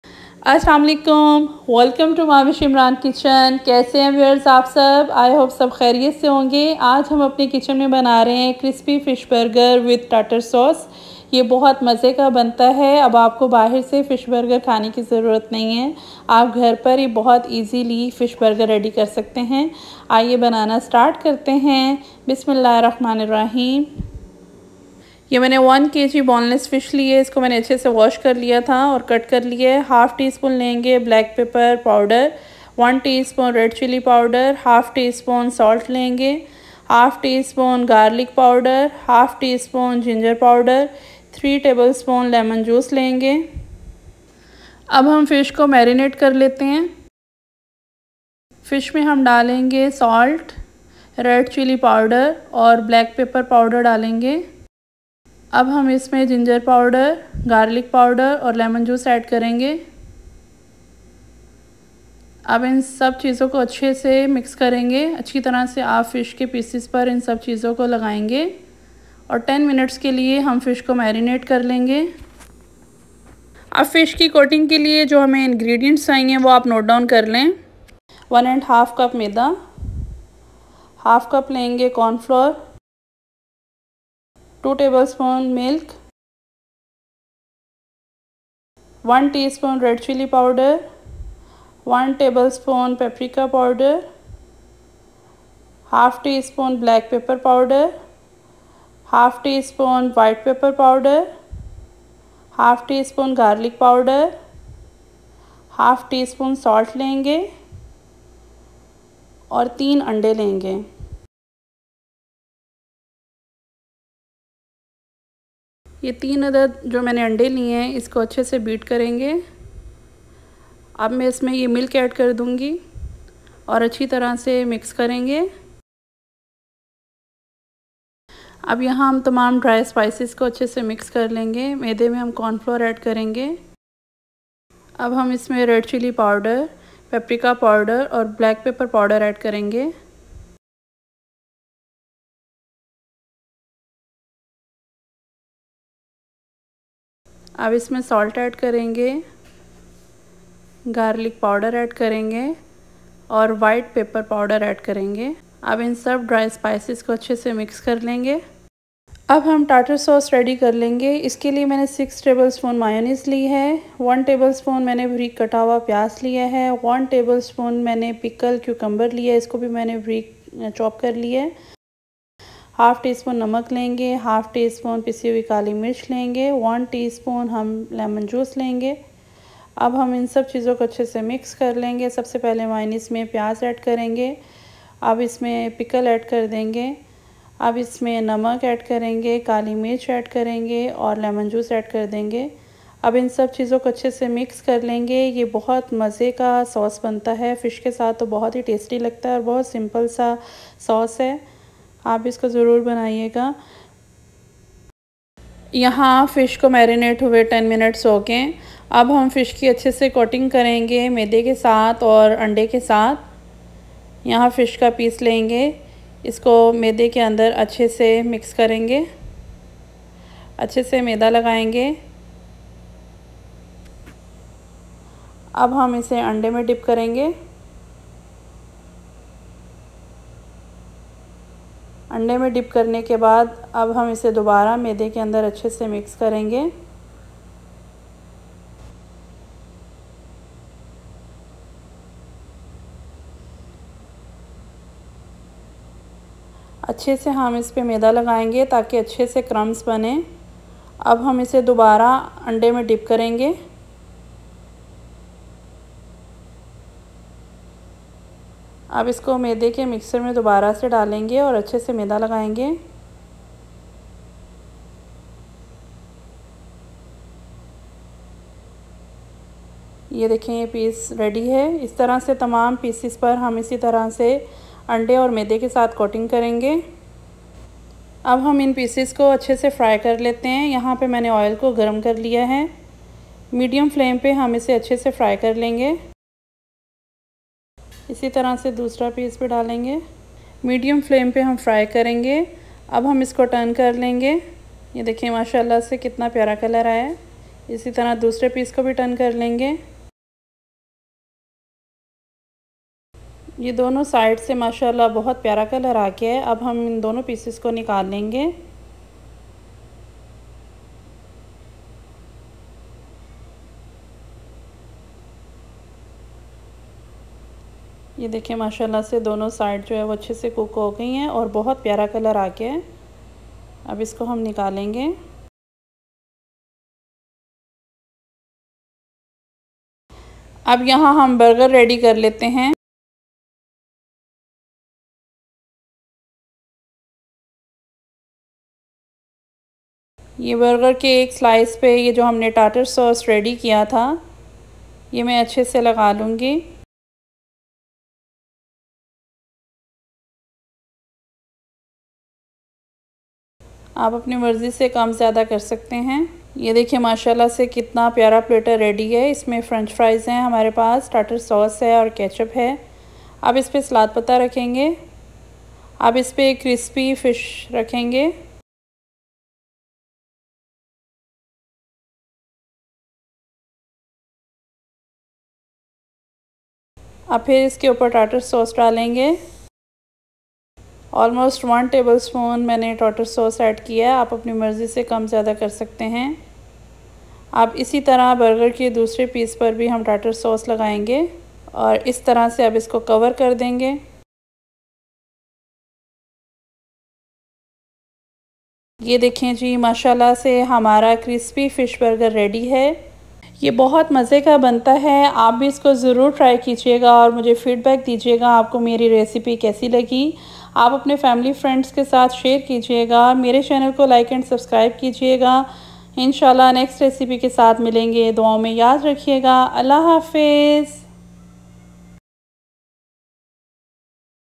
Crispy Fish Burger with Tartar Sauce by Mahwish Imran Kitchen
Today I will guide you step by step how you can make crispy fish burger with tartar sauce in your home.